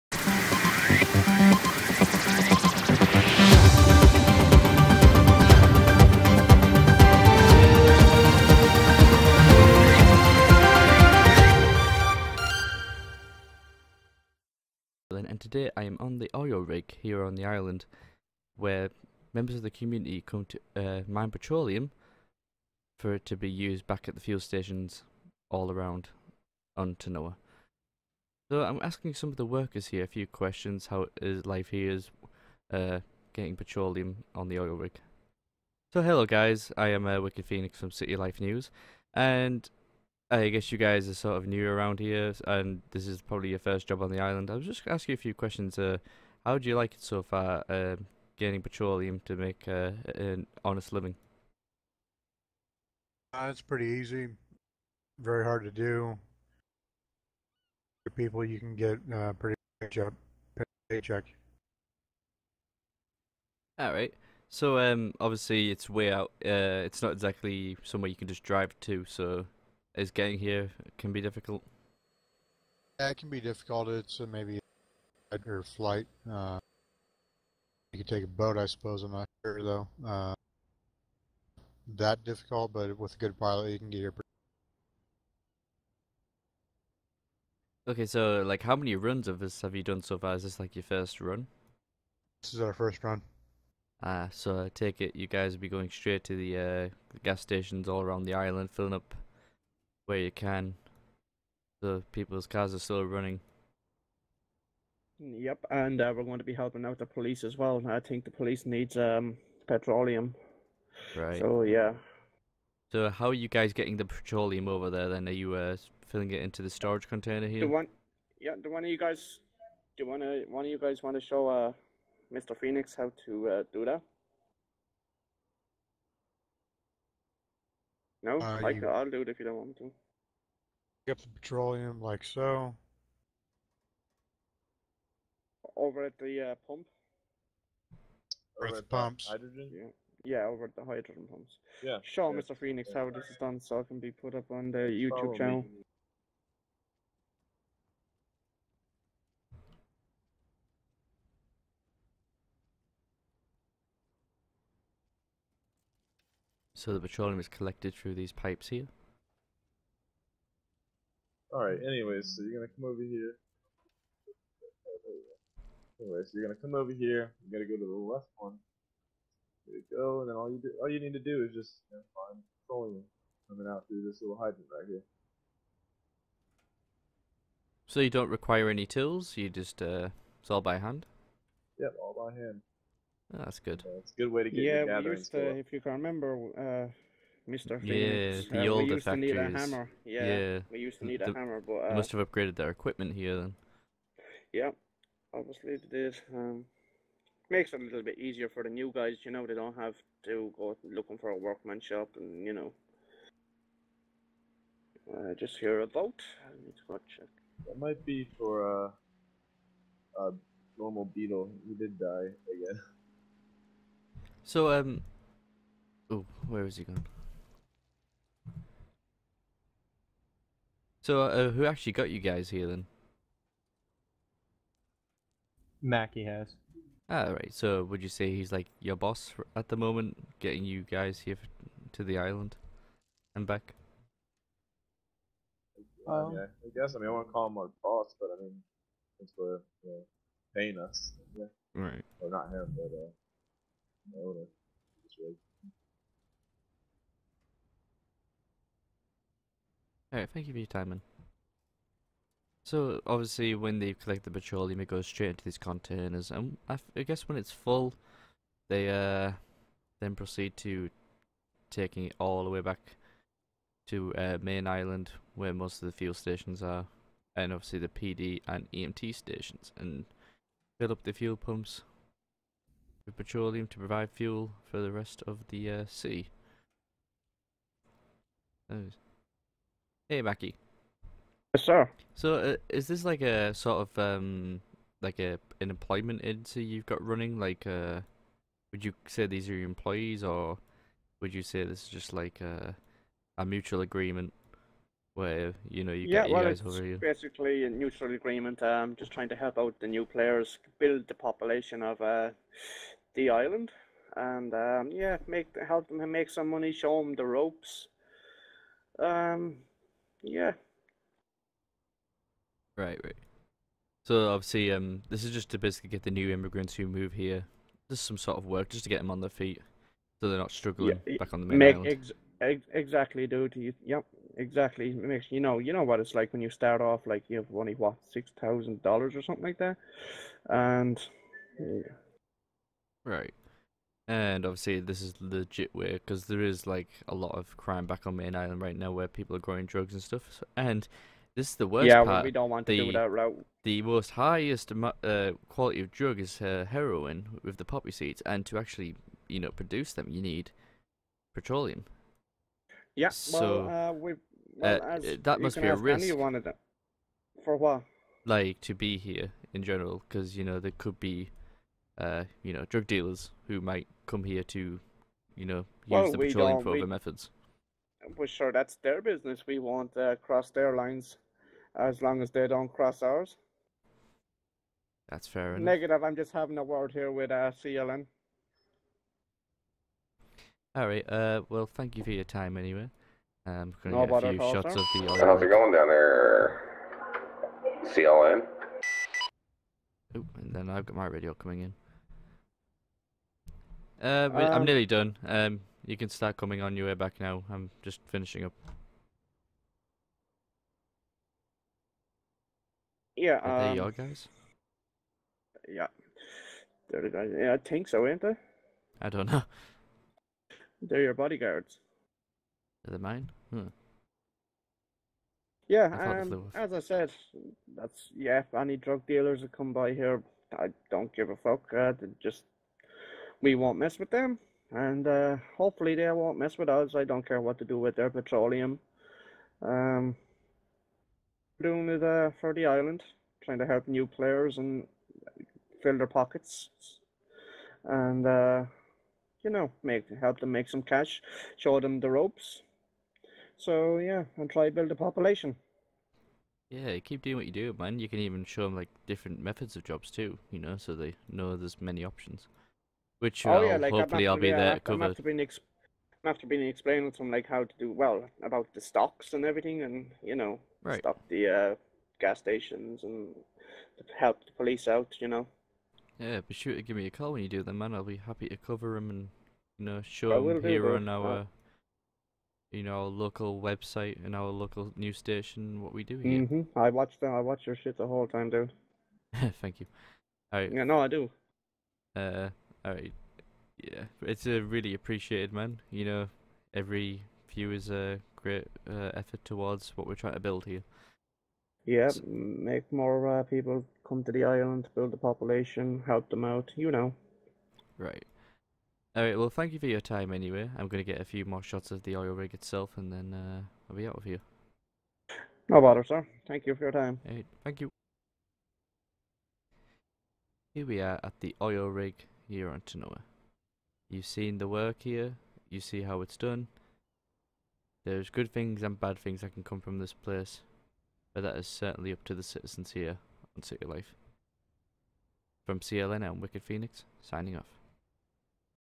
[City Life 3][CLN] Interview with Oil Rig Workers
So today we visit the oil rig to ask the civilians of city life questions about their trade.